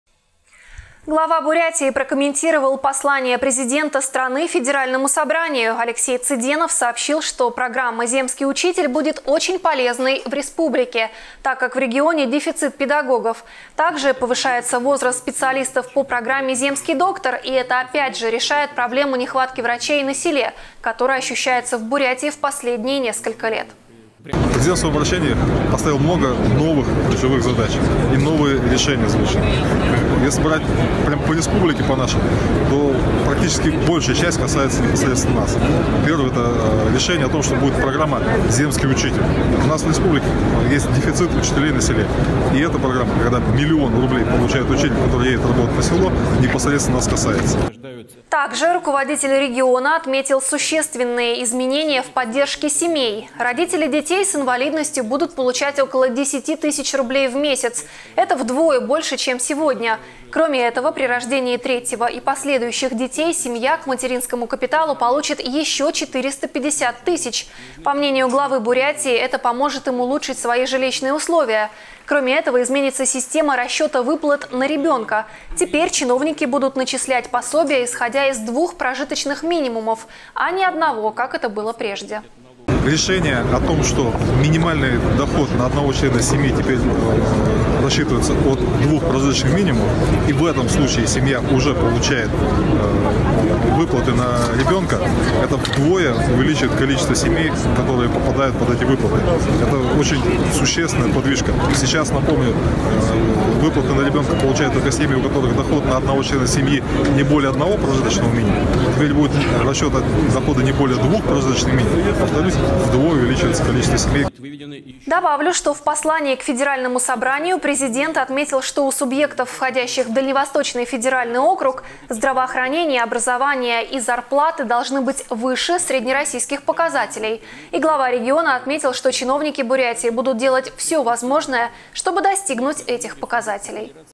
Глава Бурятии прокомментировал послание президента страны Федеральному собранию
Глава Бурятии прокомментировал послание президента страны  Федеральному собранию. Алексей Цыденов сообщил, что программа «земский учитель» будет очень полезной республике, так как в регионе дефицит педагогов.  Также повышается возраст специалистов по программе «Земский доктор». И это  опять же решает проблему нехватки врачей на селе, который ощущается в Бурятии в последние несколько лет.